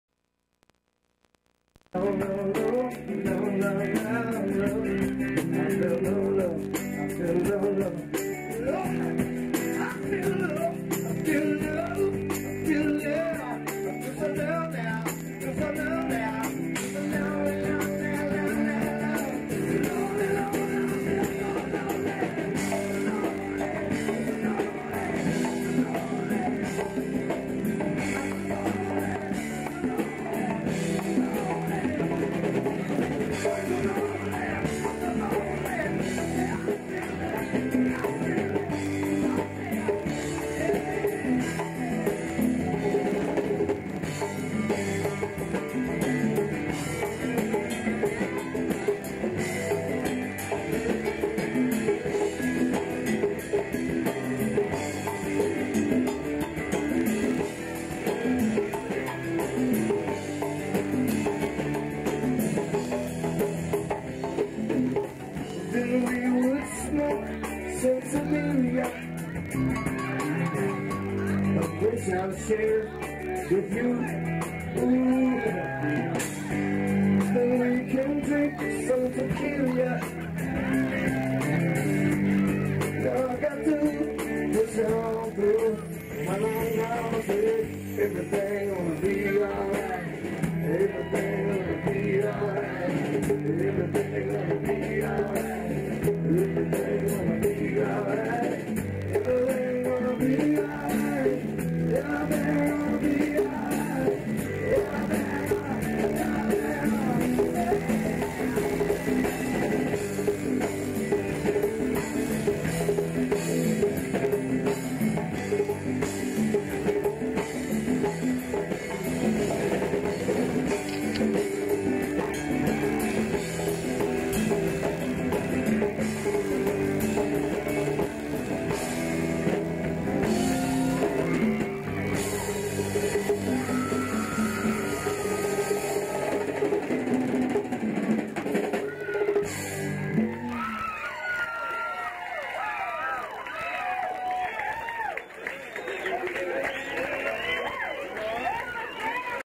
Edwin McCain and Cadillac Willy 2001
This video was shot in 2001 at Harbor Docks in Destin Florida.
With Edwin McCain, John David Sullivan, Clayton Bonjean, Patrick Wilson, Louie Antoon, Harry Eckel